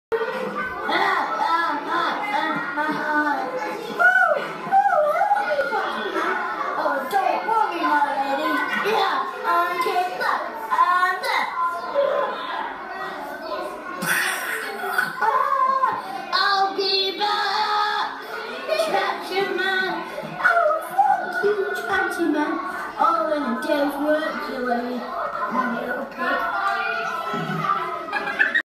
Year Two Puppet Pals
We have been reading the book Traction Man by Mini Grey. This week we used the App Puppet Pals to create our own versions with the characters we made out of kitchen utensils. We had lots of fun!